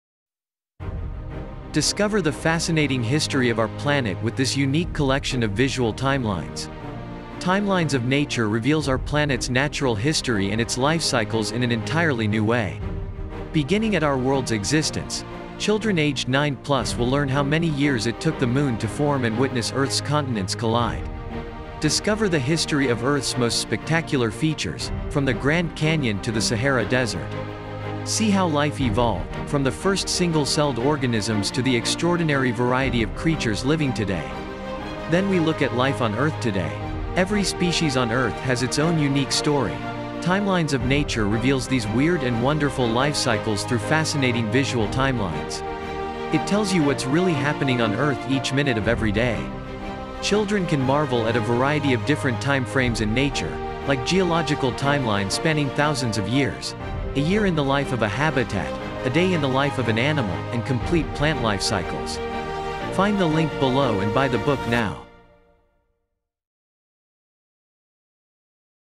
Timelines of Nature
Timelines of Nature: From Mountains and Glaciers to Mayflies and Marsupials

Discover the fascinating history of our planet with this unique collection of visual timelines.

Full of exciting visual timelines covering minutes, hours, days, weeks, months, and years, Timelines of Nature reveals our planet’s natural history and its life cycles in an entirely new way.

Beginning at our world’s existence; children aged 9+ will learn how many years it took the Moon to form and witness Earth’s continents collide. Discover the history of Earth’s most spectacular features – from the Grand Canyon to the Sahara desert. See how life evolved, from the first single-celled organisms to the extraordinary variety of creatures living today. Then we look at life on Earth today. Every species on Earth has its own unique story – Timelines of Nature reveals these weird and wonderful life cycles through fascinating visual timelines. It tells you what’s really happening on Earth each minute of every day.

This beautiful nature book for children features:

- Beautiful timelines teach young readers all about geology, plants, and animals.
- Each timeline is unique and depicts a different topic, such as the story of how whales evolved, how the Moon was formed, or how a tiger spends its day.
- Feature pages highlight climactic moments in nature, for example, the butterfly finally emerging from its chrysalis.
- Supporting educational boxes on each page explain key points about nature, helping kids to discover more about the world around them.

Children can marvel at a variety of different timeframes in nature, like geological timelines spanning thousands of years; a year in the life of a habitat, a day in the life of an animal, and complete plant life cycles.

Ever wondered what the mayfly does with its short life? In this book, you’ll find out how it lives as a nymph for two weeks, before flying out of the water for just a day in the sky as a mayfly. If you’ve ever thought about how a chimpanzee spends its day, how the rainforest changes over 24 hours, or how long a kangaroo’s pregnancy lasts, then this is the book for you!